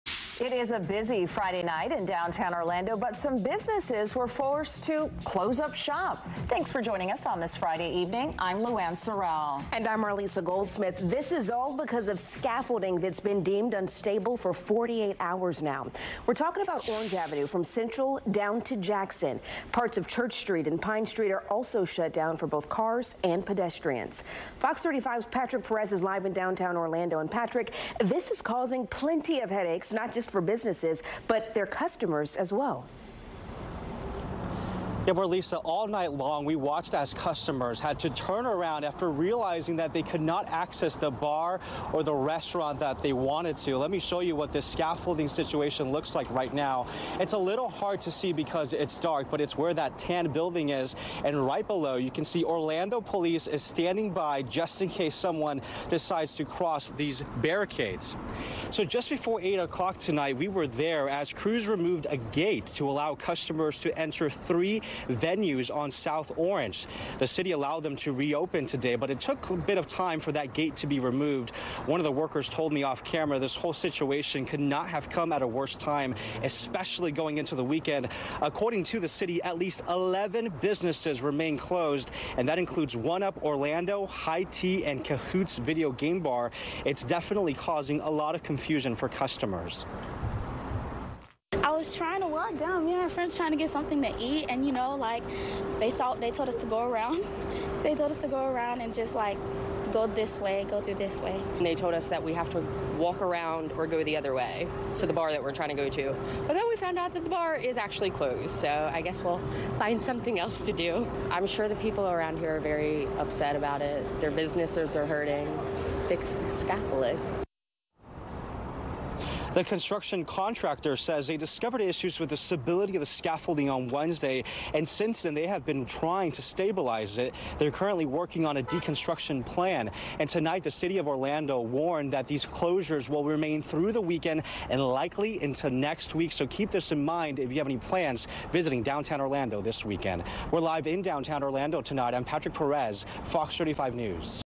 11 Downtown Orlando businesses forced to temporarily close
Some businesses in downtown Orlando have been forced as a result of unstable scaffolding in the area.